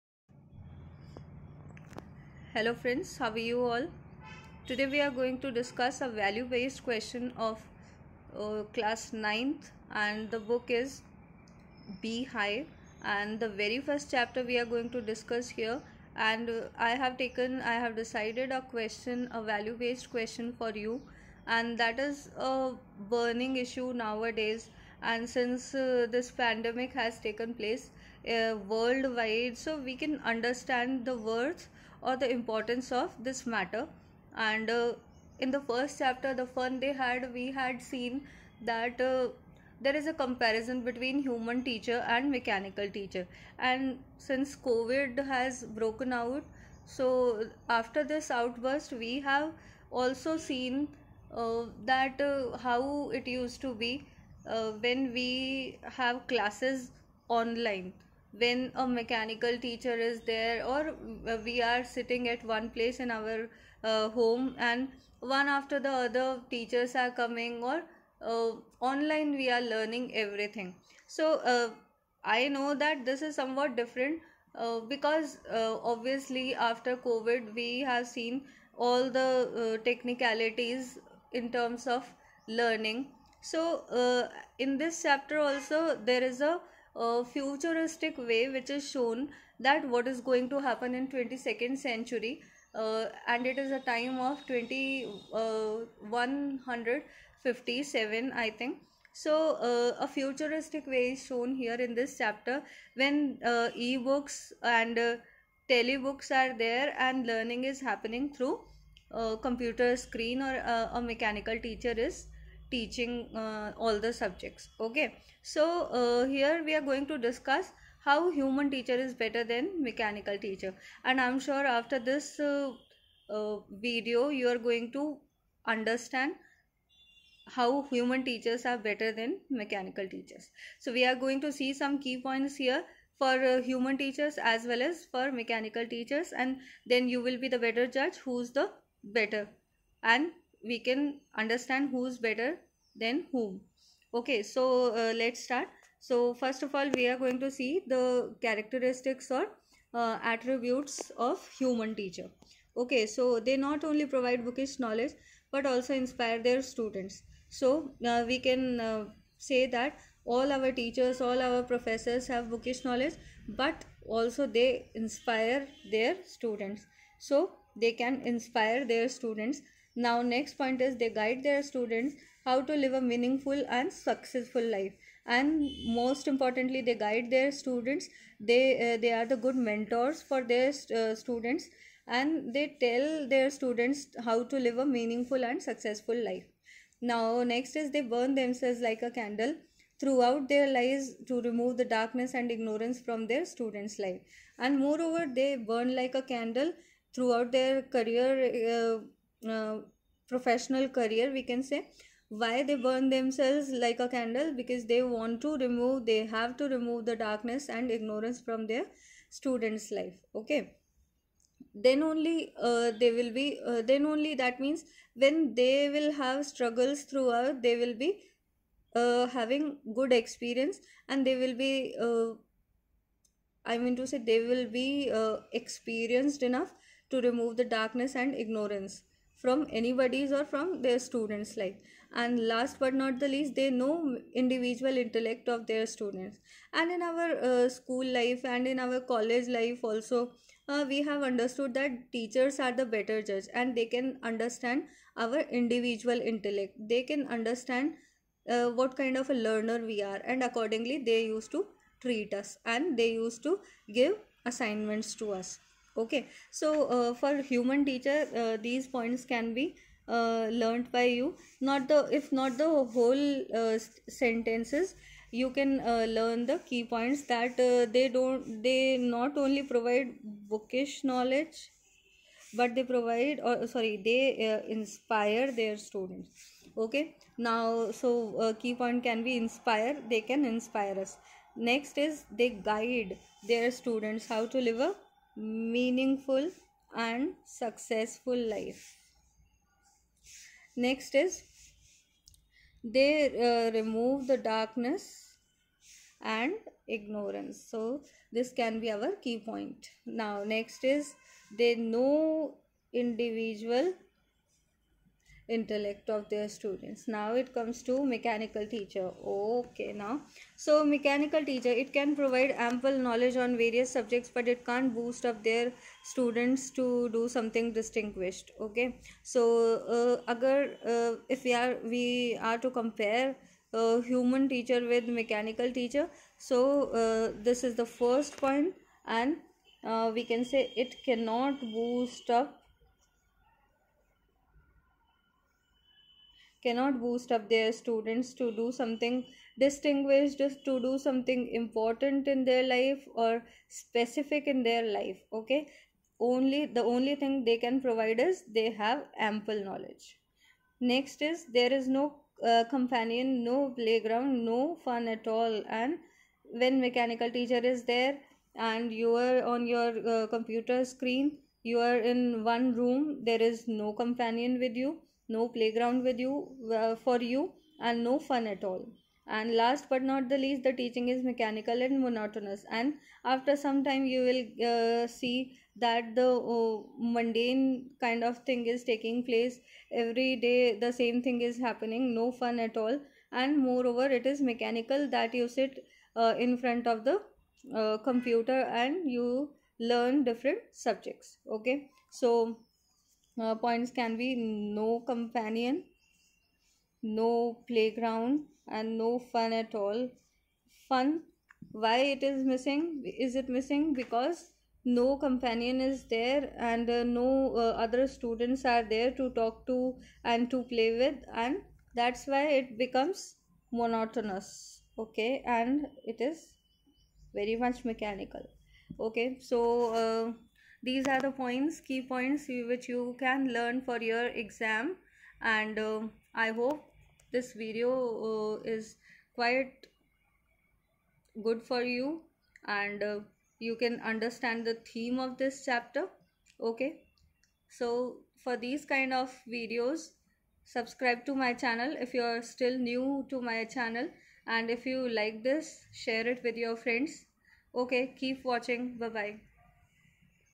The Fun They Had l Difference between Human Teacher and Mechanical Teacher l Value Based que l Cls 9
Hi friends
I welcome u all on our YouTube channel